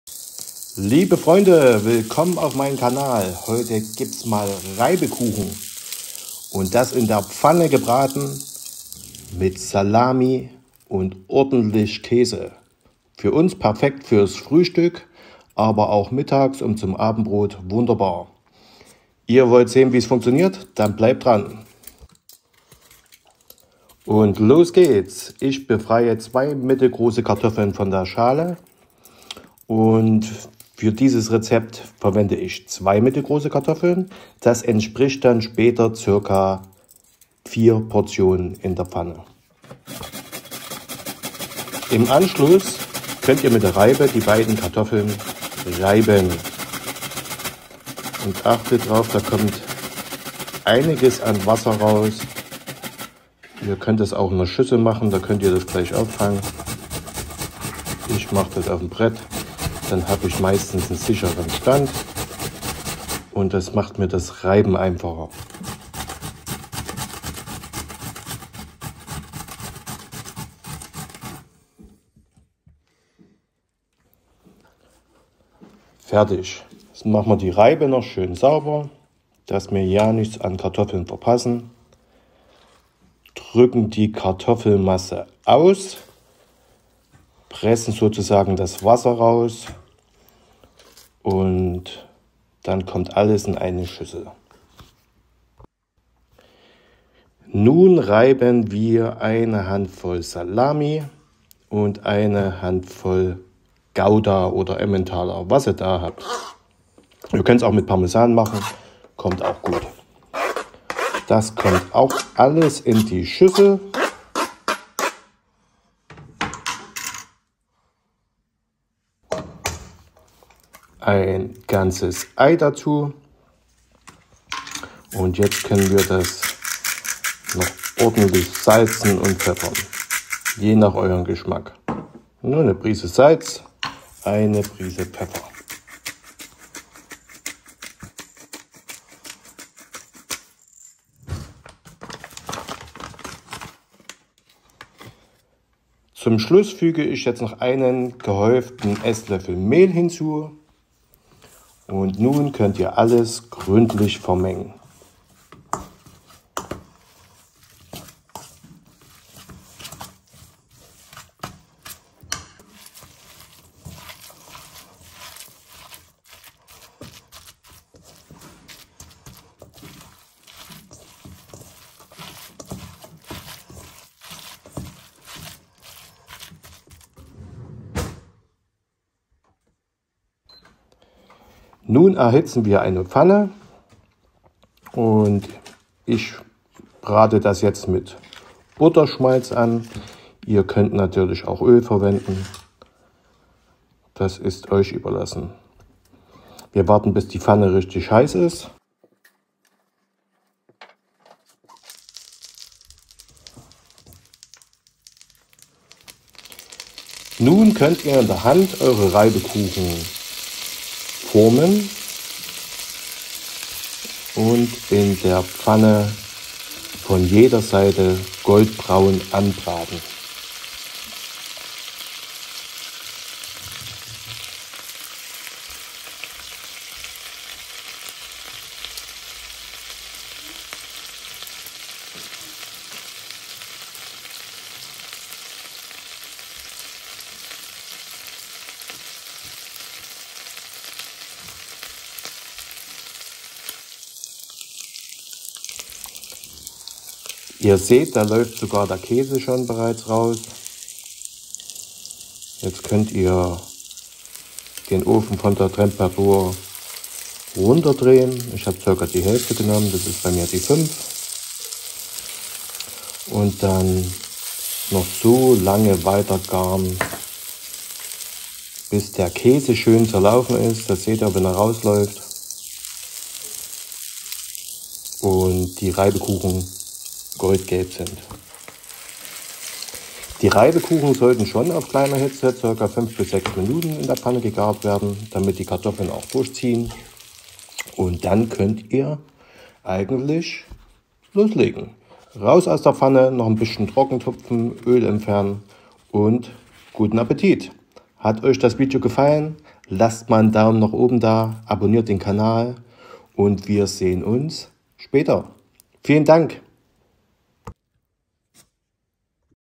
Habt ihr Kartoffeln, Salami und Käse da… dann gibts Reibekuchen vom feinsten… Frittata
Reibekuchen mit Salami und Käse, wunderbar für Frühstück aber auch als Snack... Deftig zum Abendbrot. Leckeres Frittata
Zutatenfür 4 Reibekuchen:
2 Kartoffeln
1 Ei
1 Hand voll Salamie
1 Hand voll Käse (Gauda, Ementaler, Parmesan oder Cheddar)
1 EL Mehl

Guten Appetit